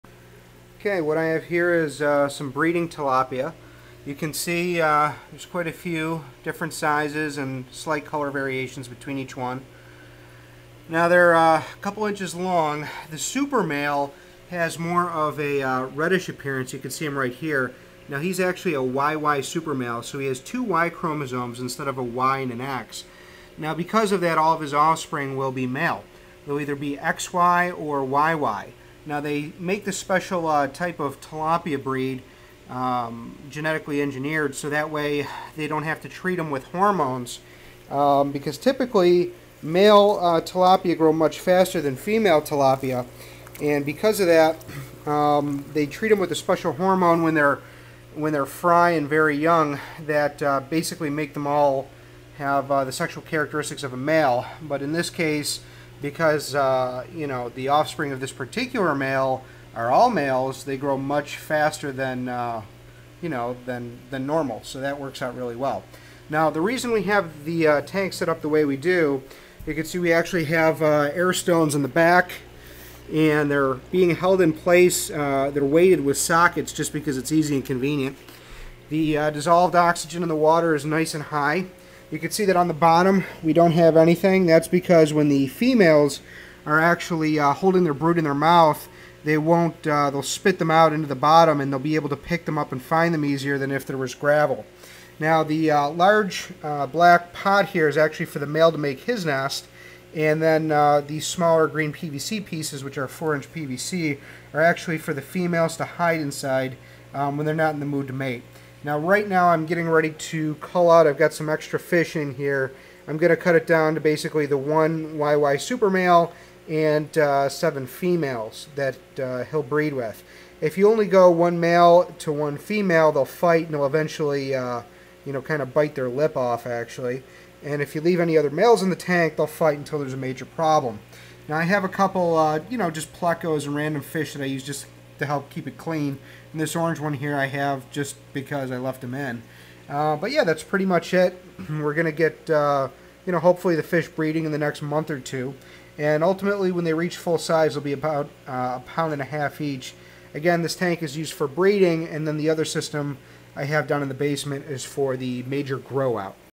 Tilapia Farming And Breeding Tank System
This video covers proper setup for tilapia breeding, and also features a YY supermale tilapia as well.